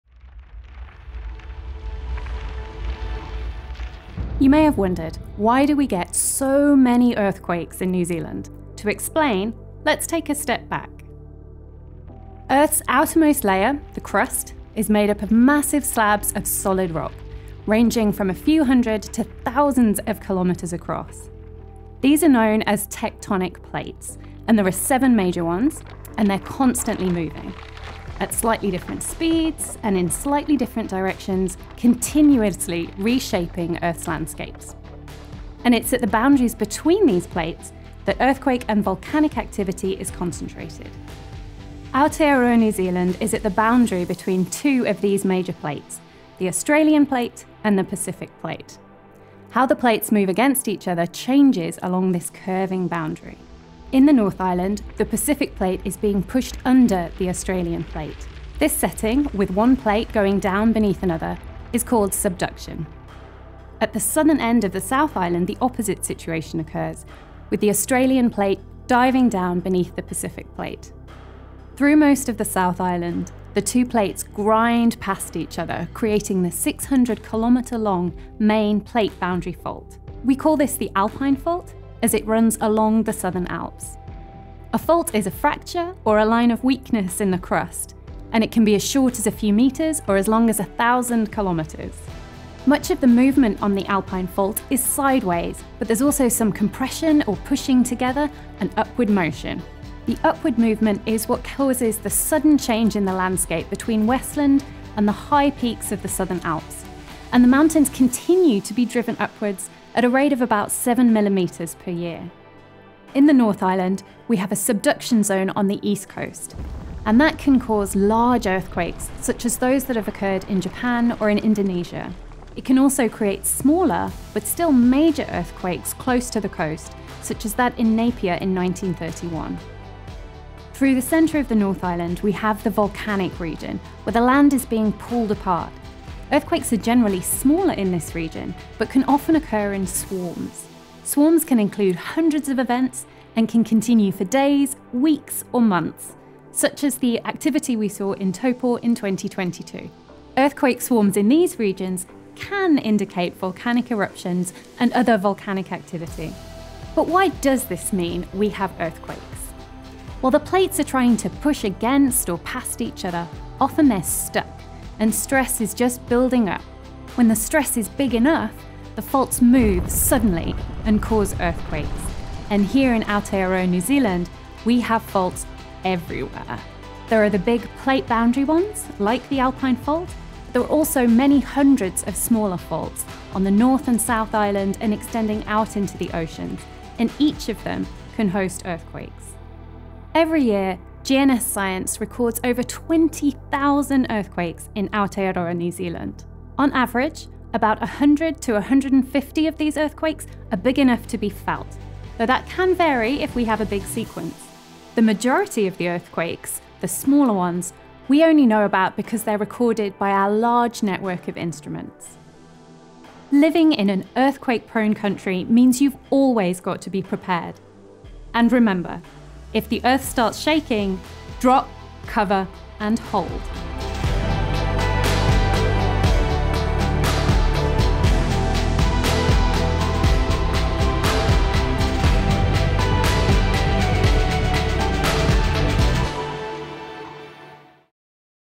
Earthquakes in Aotearoa New Zealand
On average, we locate over 20,000 earthquakes a year in and around Aotearoa New Zealand. So why do we get so many earthquakes? Discover the cause, and what is happening beneath our feet.